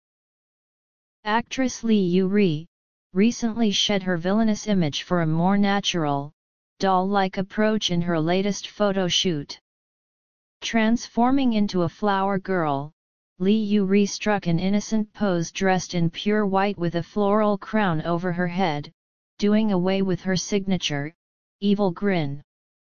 Lee Yoo Ri sheds her villainous image for latest photoshoot
GOT7's Jackson poses for the Chinese edition of 'Grazia'

Lee Yoo Ri sheds her villainous image for new photoshoot

Actress Lee Yoo Ri recently shed her villainous image for a more natural, doll-like approach in her latest photo shoot!

Transforming into a flower girl, Lee Yoo Ri struck an innocent pose dressed in pure white with a floral crown over her head, doing away with her signature, evil grin.